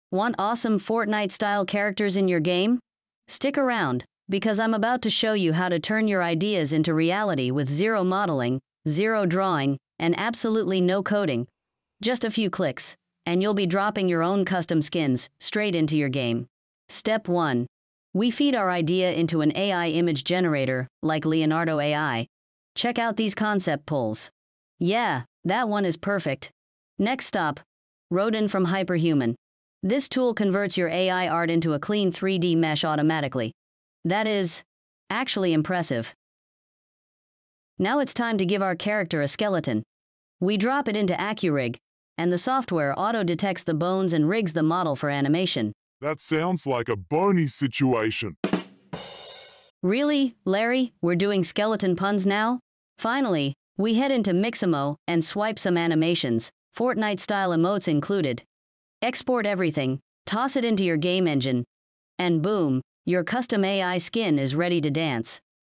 Create Your Own Game Character in Minutes!
Want to make your own Fortnite-style characters for your game? In this short, I show how I used AI tools to generate, convert, rig, and animate a custom Halloween skeleton skin—and drop it straight into a game engine with zero modeling or coding.

🔥 More daily AI + Game Dev tricks coming—drop a comment if you want me to turn this into a full character pack!